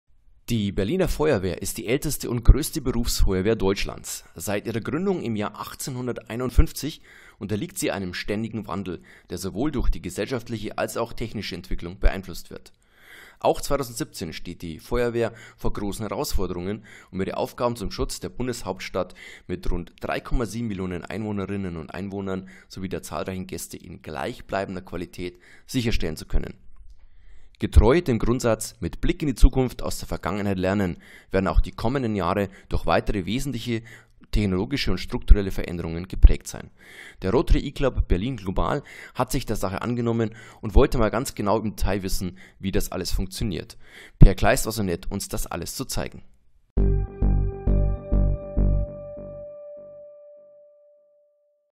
Besuch bei der Berliner Feuerwehr.
Die Berliner Feuerwehr ist die älteste und größte Berufsfeuerwehr Deutschlands. Seit ihrer Gründung im Jahr 1851 unterliegt sie einem ständigen Wandel, der sowohl durch die gesellschaftliche als auch technische Entwicklung beeinflusst wird. Auch im Jahr 2017 steht die Berliner Feuerwehr vor großen Herausforderungen, um ihre Aufgaben zum Schutz der Bundeshauptstadt mit ihren fast 3,7 Mio. Einwohnerinnen und Einwohnern sowie der zahlreichen Gäste in gleichbleibender Qualität sicherstellen zu können. Getreu dem Grundsatz "Mit Blick in die Zukunft aus der Vergangenheit lernen" werden auch die kommenden Jahre durch weitere wesentliche strukturelle und technologische Veränderungen geprägt sein.

The Berlin Fire Brigade is the oldest and largest professional Fire Brigade in Germany. Since its founding in 1851, it has been subject to constant change, influenced both by social and technological development. In 2017, the Berlin Fire Brigade will again face major challenges in order to be able to ensure its tasks to protect the federal capital with its almost 3.7 million inhabitants and the numerous guests. True to the basic principle "Learning from the past with a view to the future", the coming years will also be marked by further significant structural and technological changes.